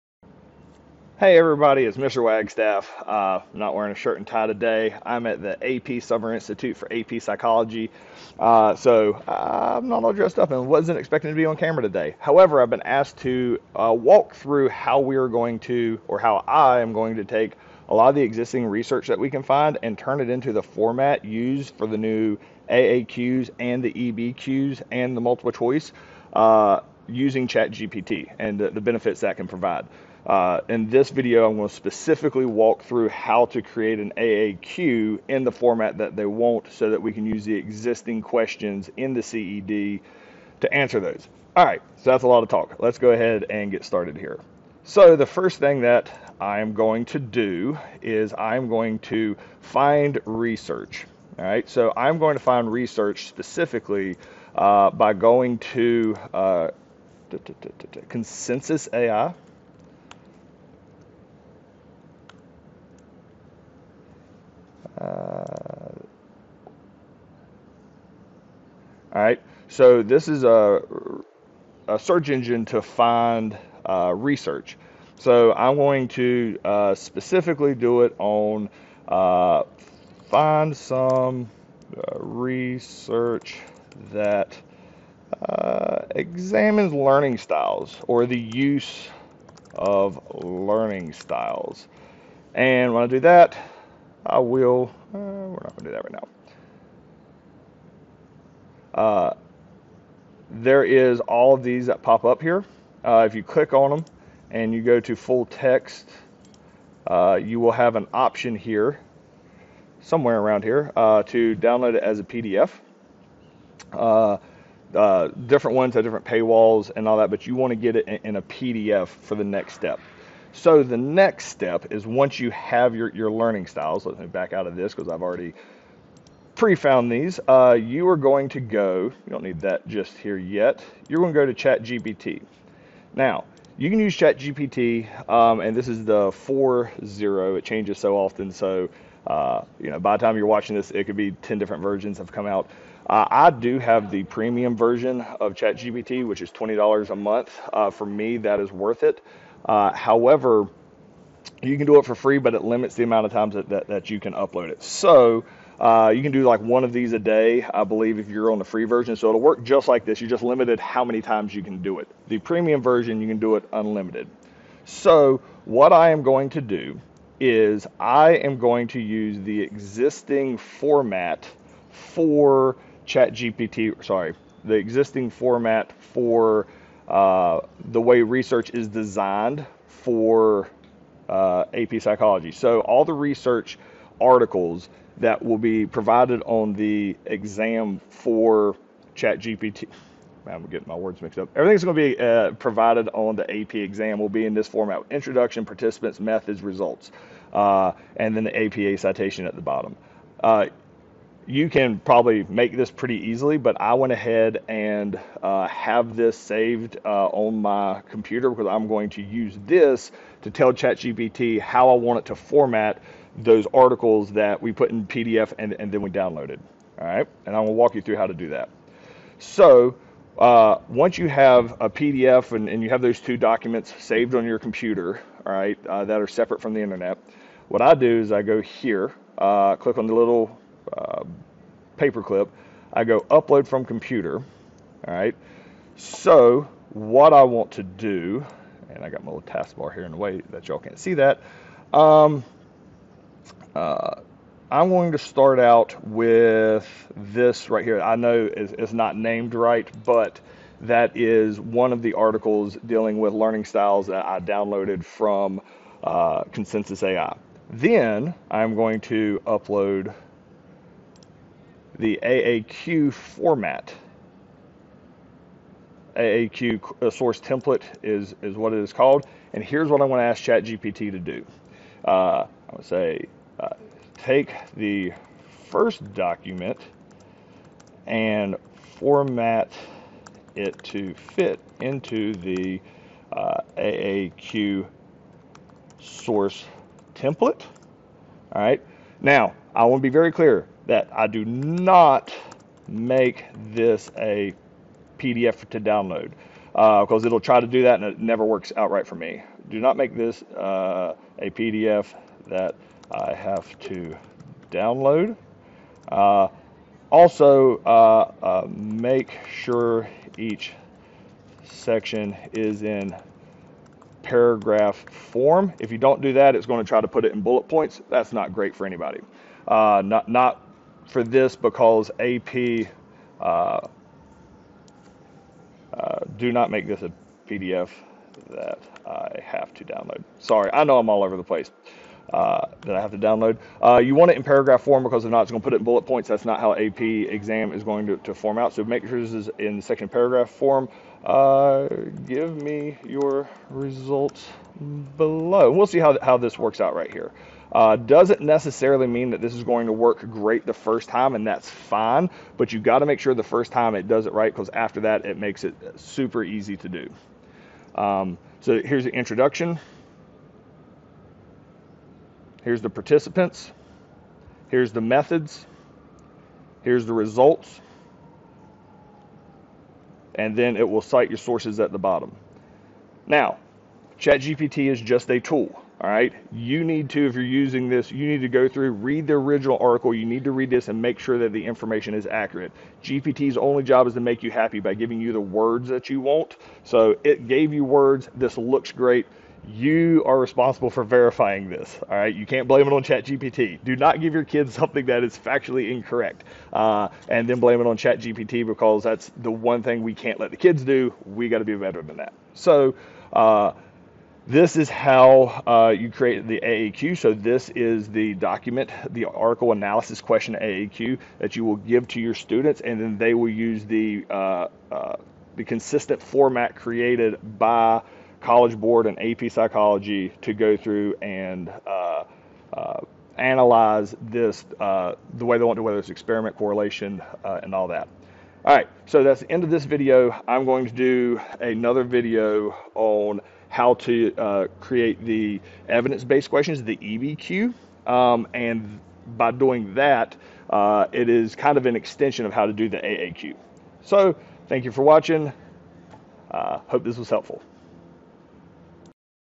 AAQ using ChatGpt Tutorial
This is a walk through of how to use ChatGPT and Consensus AI to create AAQ (Article Analysis Questions) for AP Psychology Students using the new 2024 CED requirements.